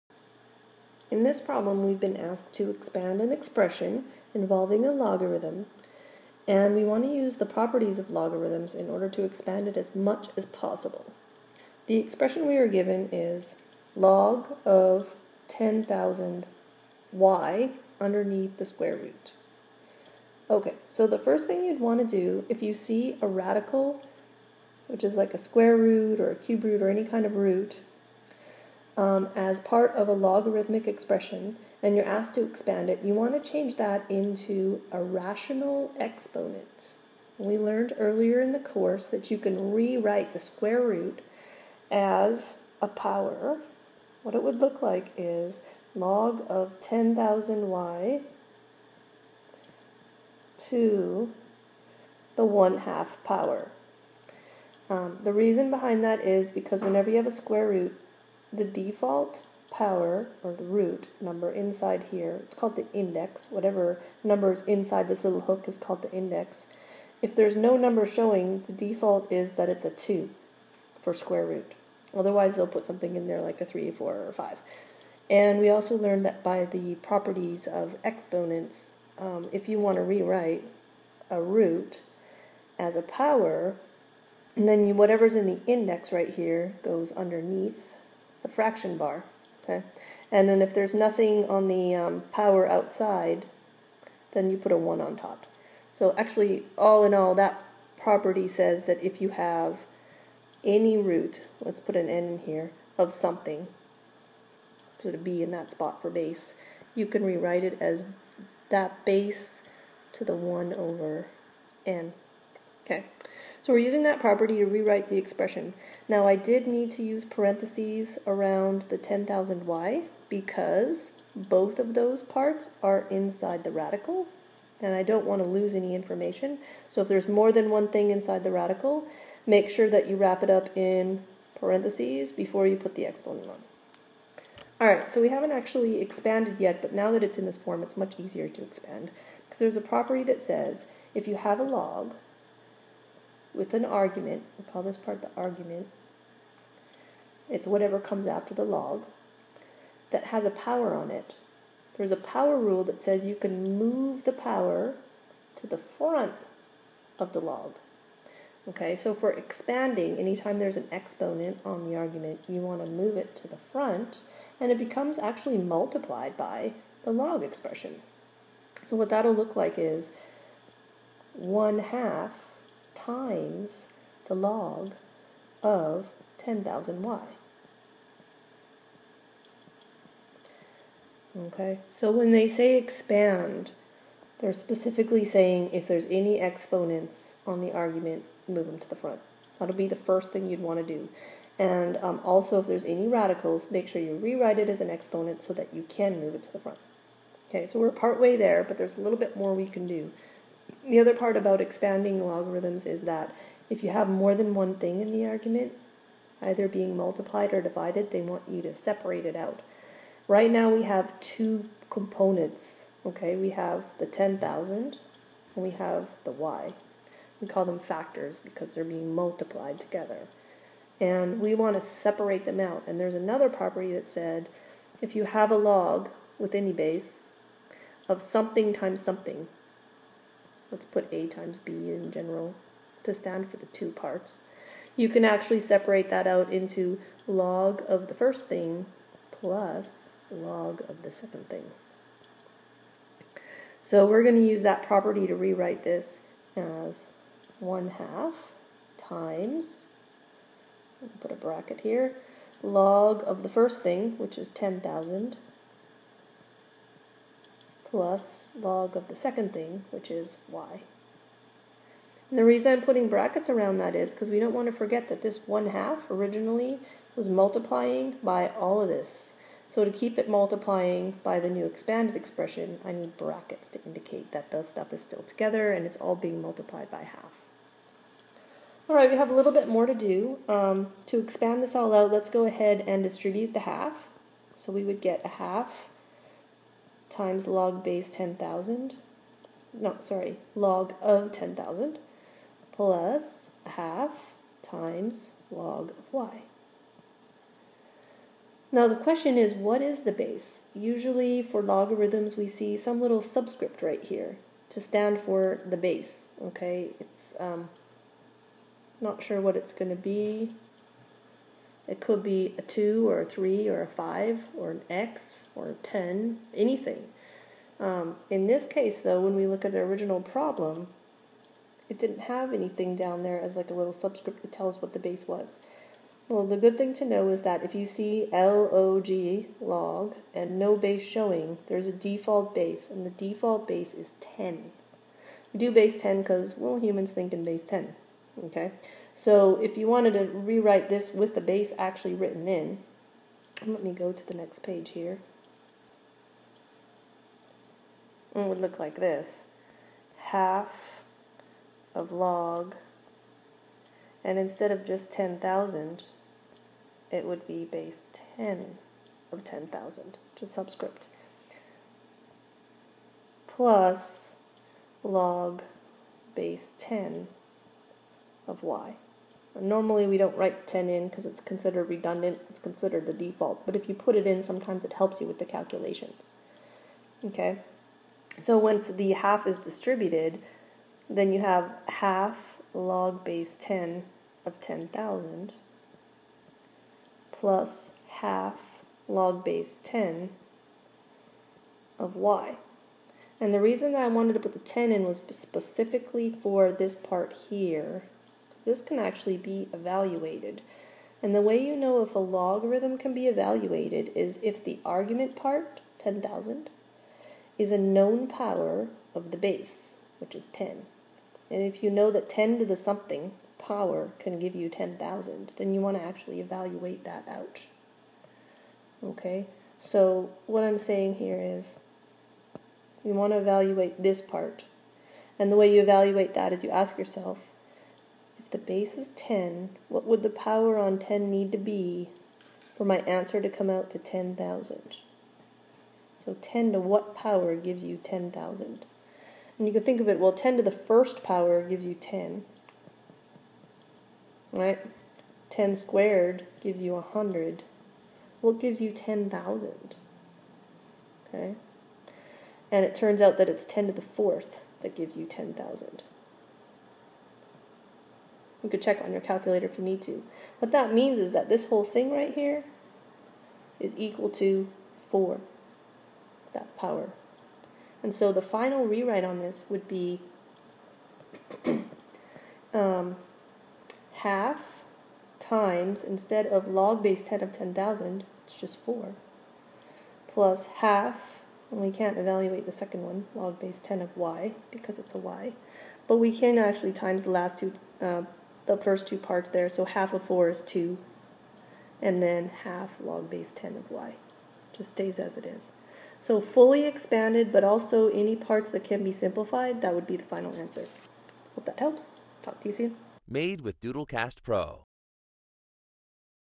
Expand log base 10 with square root
How to fully expand and simplify a log in base 10 with a square root